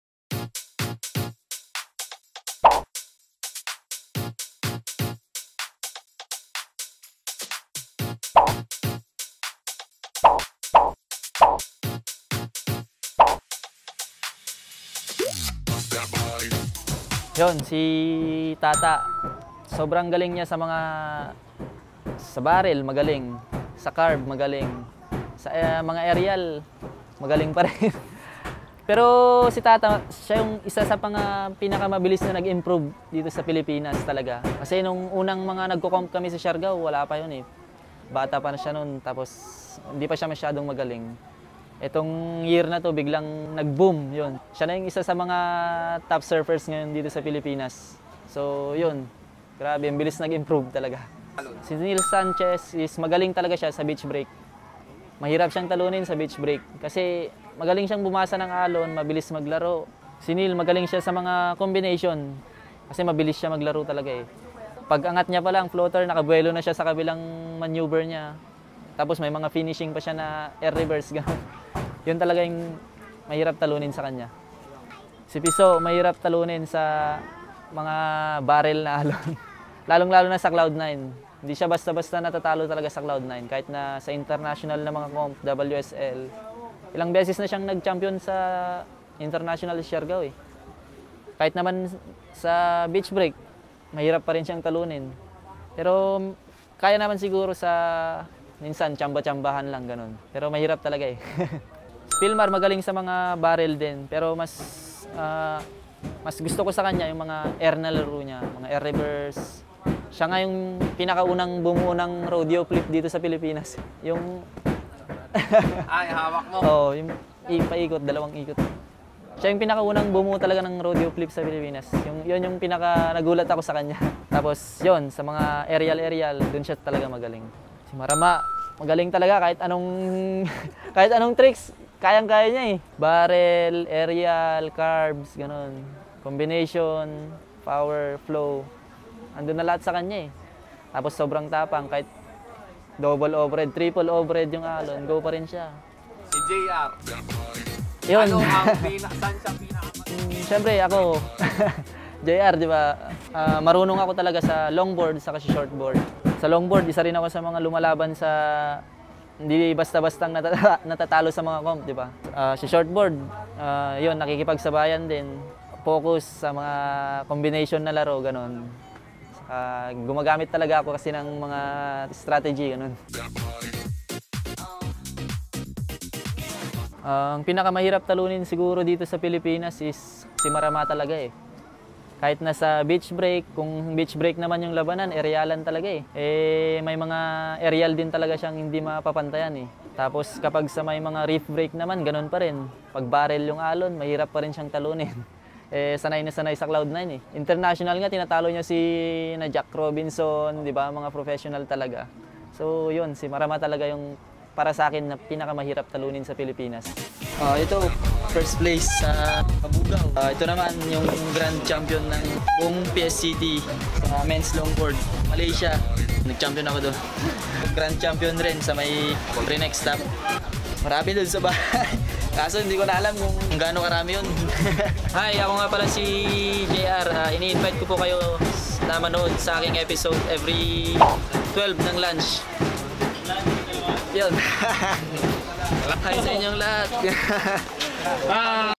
Jay r Esquivel Part 3 Agiants The Pros
We asked the best surfer from La Union to describe the top surfers of the Philippines. This is Jay-r Esquivel Against the Pros